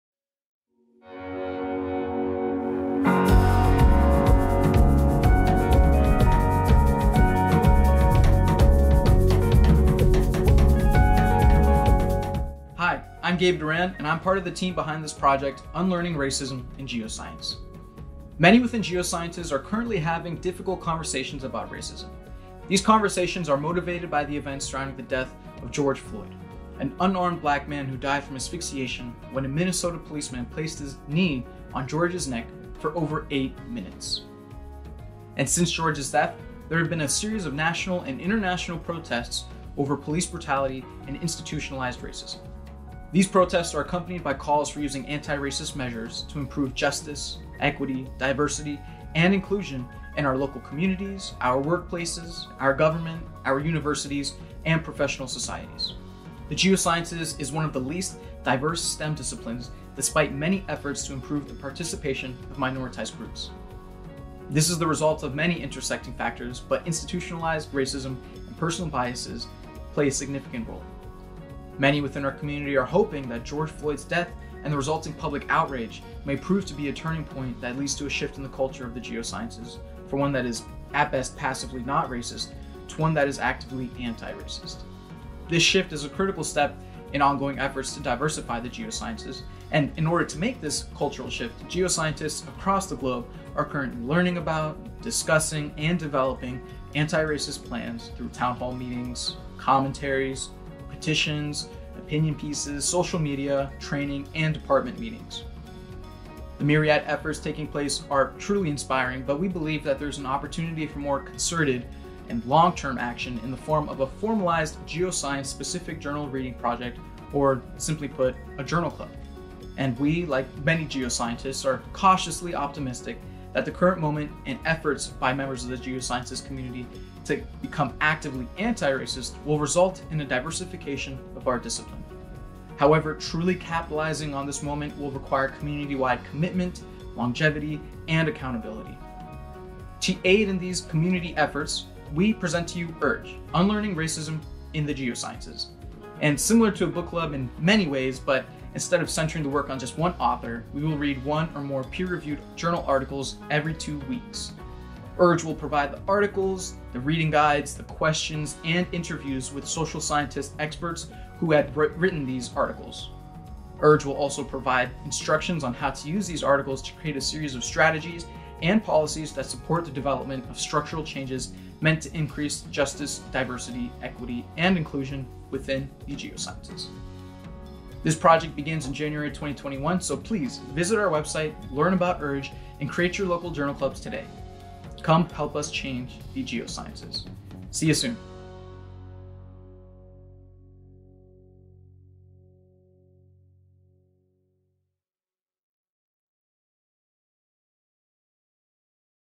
URGE Intro Video
URGE’s primary objectives are to (1) deepen the community’s knowledge of the effects of racism on the participation and retention of black, brown, and indigenous people in Geoscience, (2) use the existing literature, expert opinion, and personal experiences to develop anti-racist policies and strategies, and (3) share, discuss, and modify anti-racist policies and strategies within a dynamic community network and on a national stage. By meeting these objectives, we hope that Geoscience departments and societies will be able to implement a series of well-researched crowdsourced anti-racist policies.